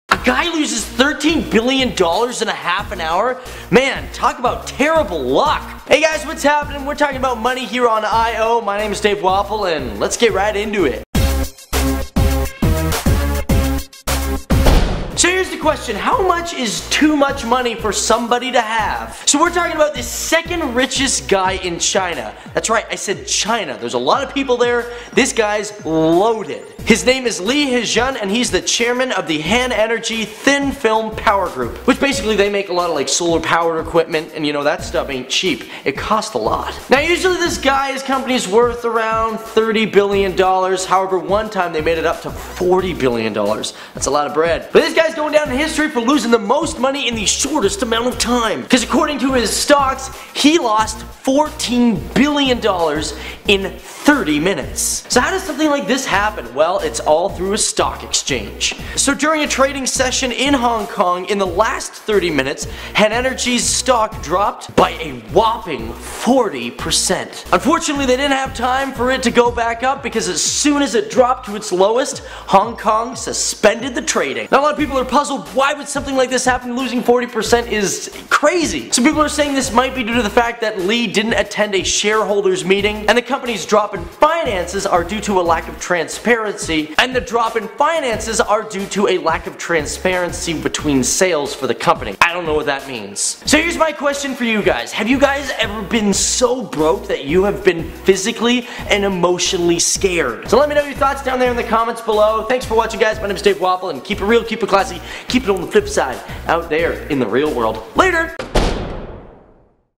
Business Man Loses $14 Billion In 30 Mins
A businessman lost almost half his fortune in 30 minutes.
Chinese billionaire Li Hejun was previously known as the second-richest man in China.

2408 Lakeshore Blvd W.